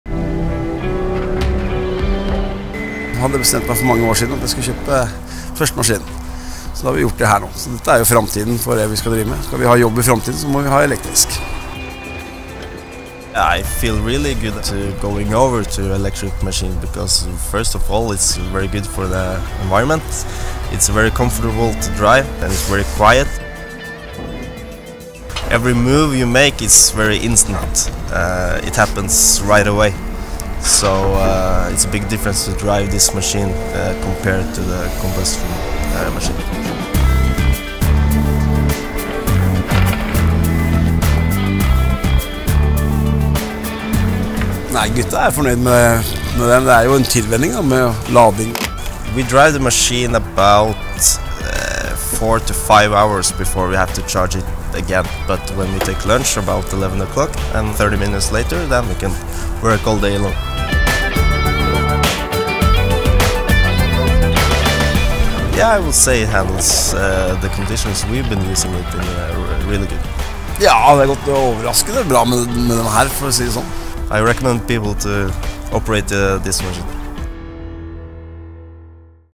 Volvo EC230 Electric: Customer Testimonial
Watch how the 23-ton electric-powered crawler excavator, Volvo EC230 Electric, performs on site. The Customer Walan Maskin AS in Oslo, Norway, understand the benefits that electric solutions add to their business. The EC230 Electric delivers instant torque, near silence, and speedy charging, handling this site's conditions with ease.

“If we want jobs in the future, we must have electric machines.” - Christian and his team at Walan Maskin AS, Oslo, Norway.